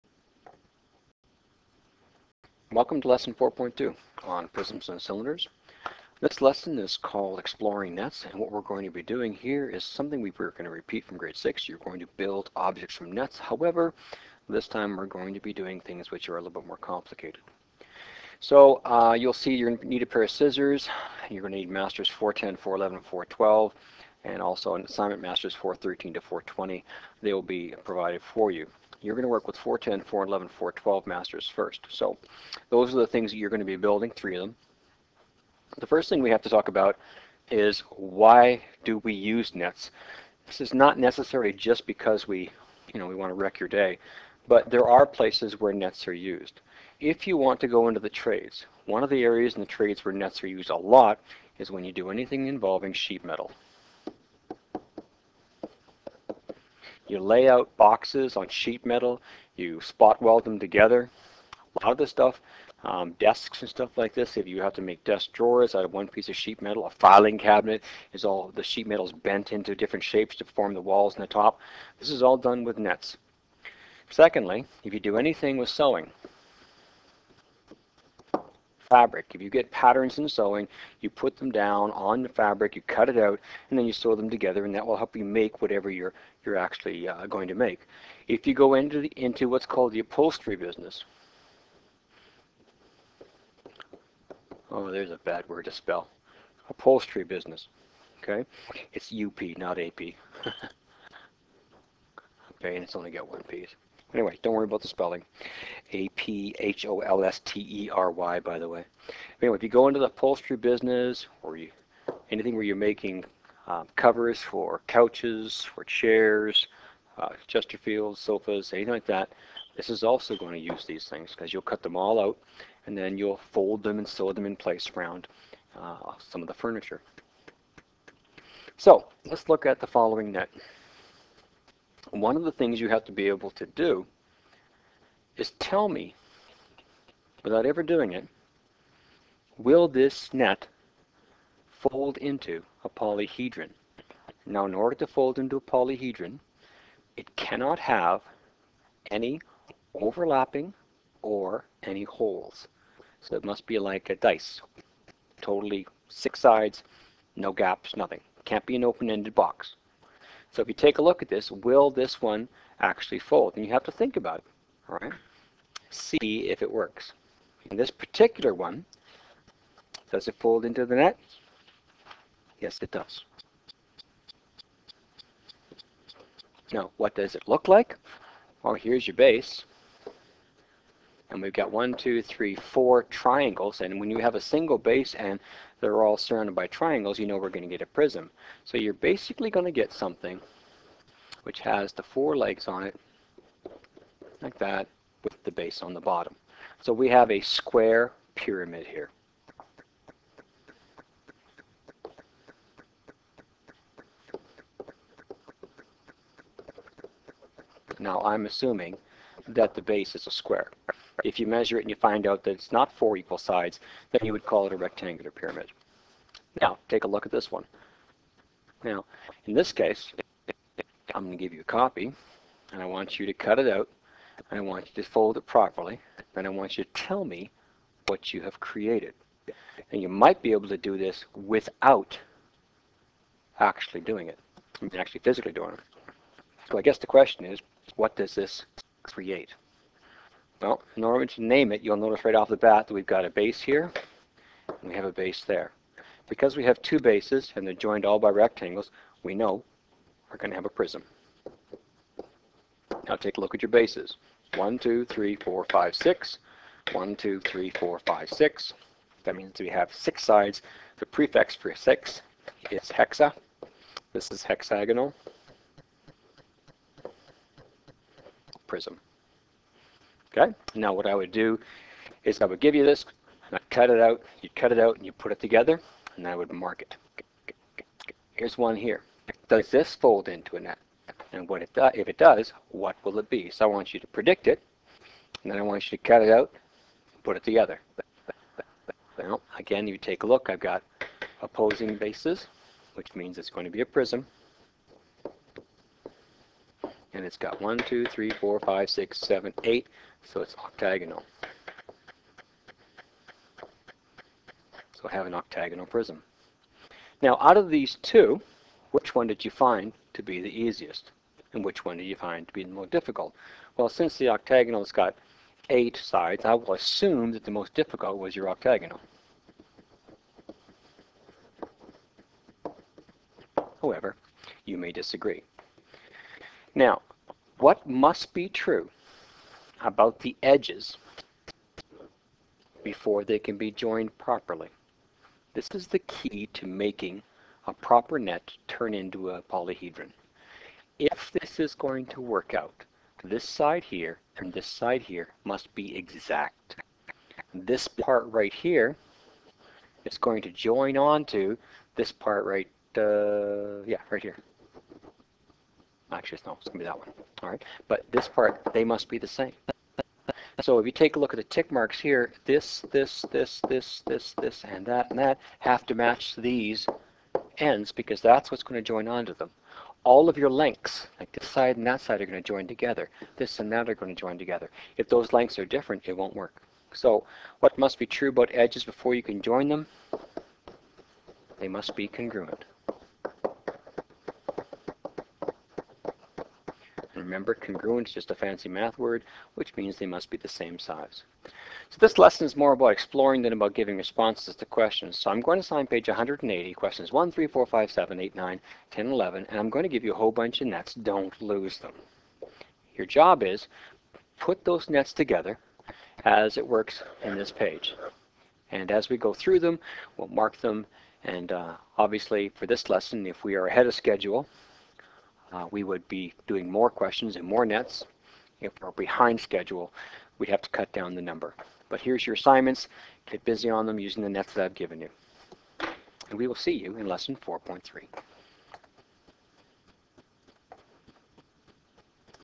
Grade 8 Math Lesson 4.2 Creating Objects From Nets
Students will be given a net which they have to cut out and then fold into a 3D object. We are restricting to right prisms and right pyramids.